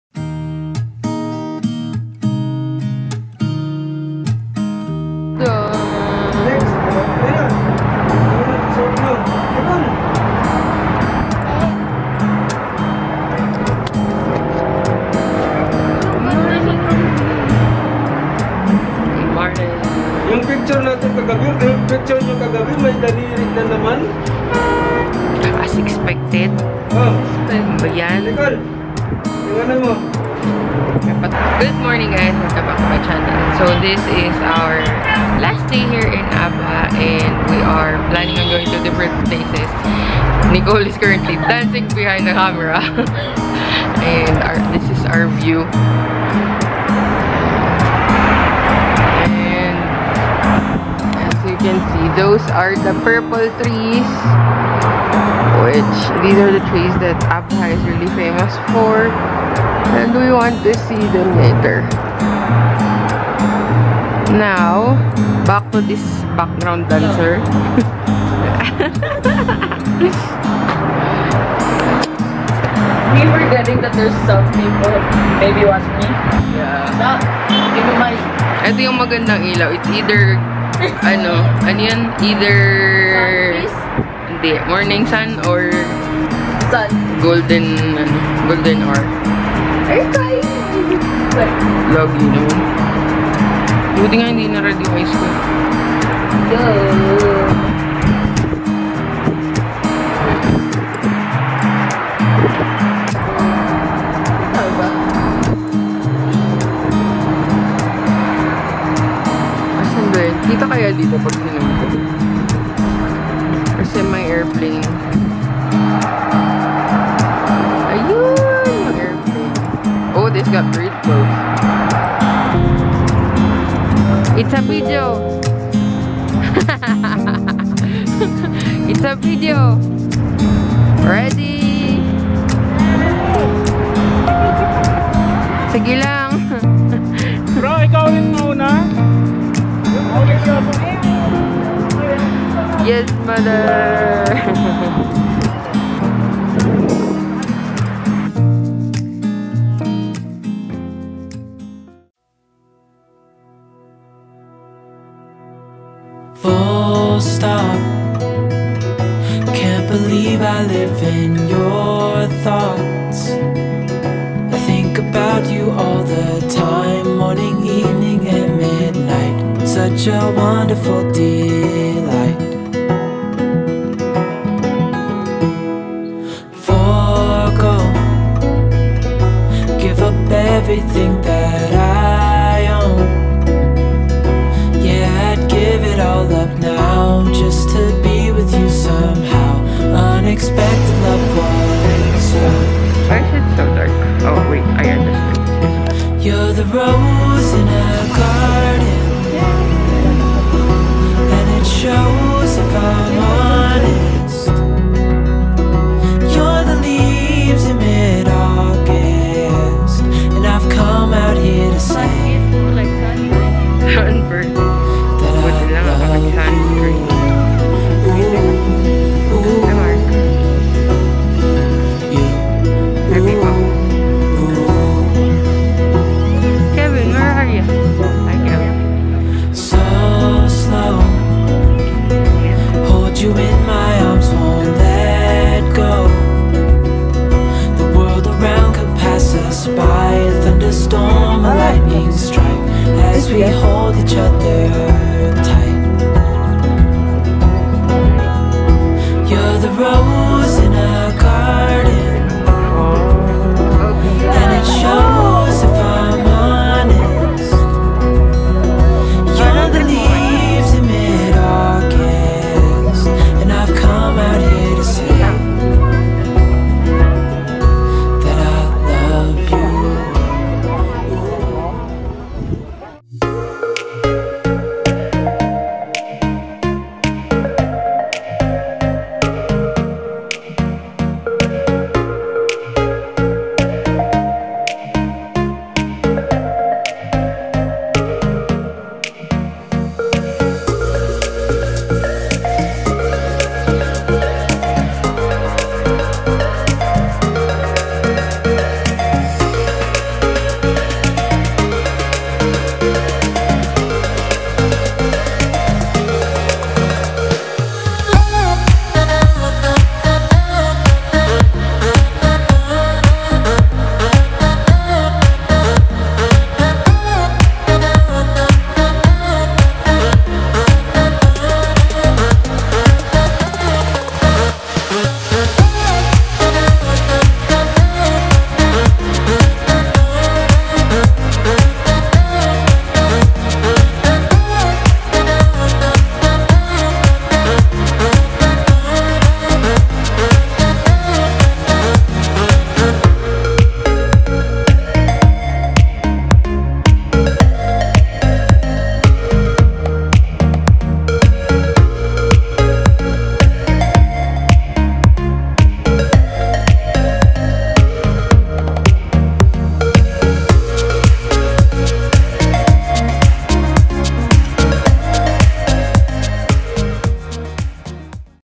Tript to Abha (Part 2) - Abha's Purple Park - MAY 2021
Hey this is Aya! Thanks for dropping by my channel!

During our Eid Al Fitr holiday, we decided to go on a trip to Abha. Although it didn't go as we planned, we enjoyed the experience nonetheless.

Unfortunately, we weren't able to go around Abha as much as we wanted to. It was basically a series of unfortunate events. That being said, we were thankful for seeing what we saw.

Editing software used:
-Filmora 9
-Audacity

Cameras Used: Canon Powershot  G7x Mark II for Videos